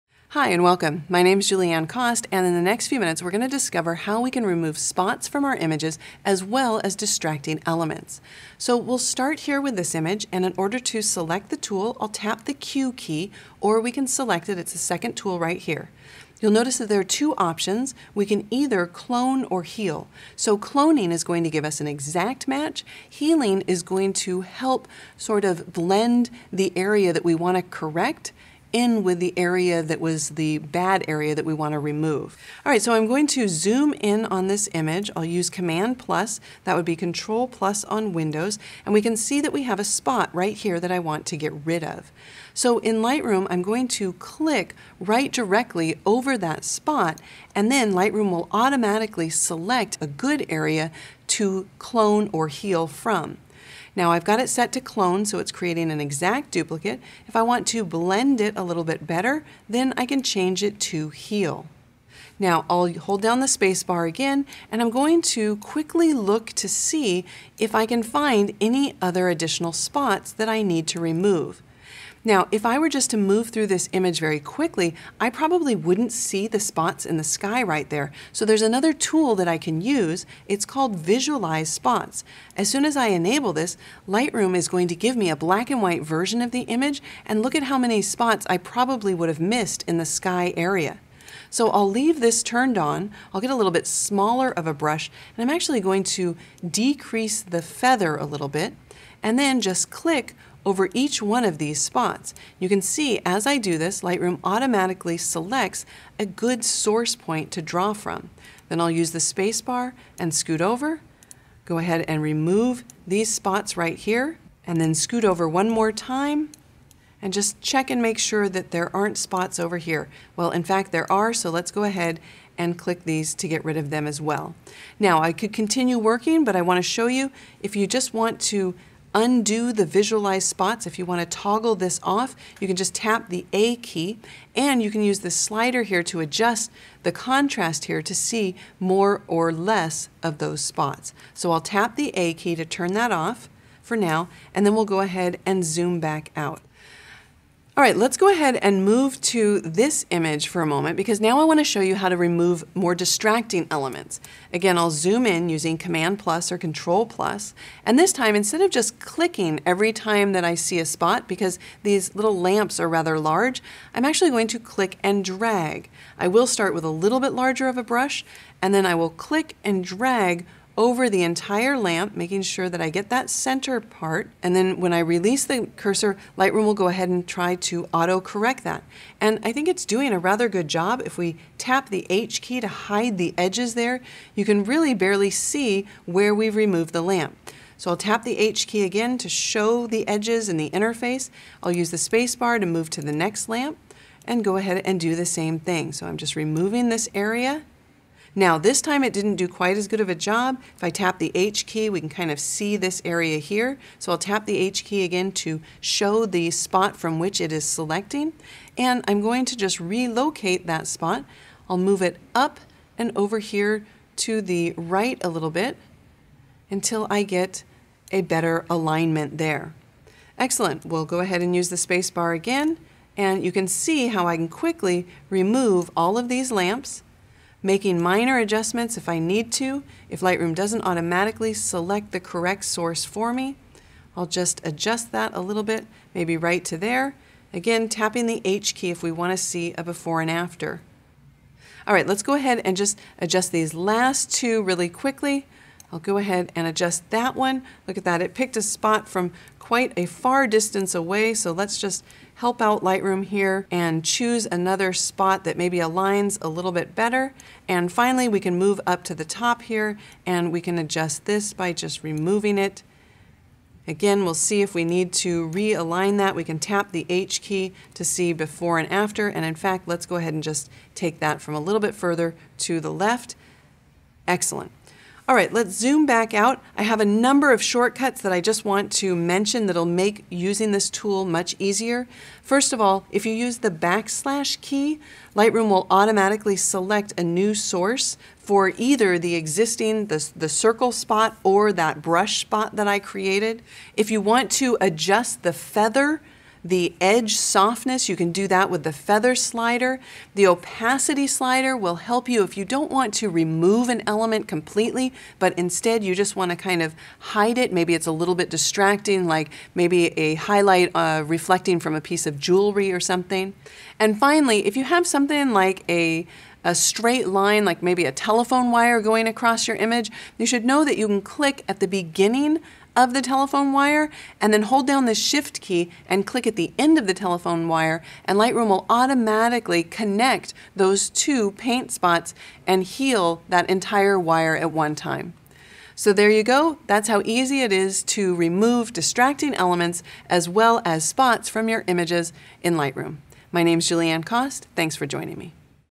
Lightroom CC - Removing Dust Spots and Imperfections | Adobe Lightroom
*The product featured in this video is Lightroom Classic CC, previously known as Lightroom CC.

Discover how to quickly remove dust spots, imperfections and distracting elements in a photograph.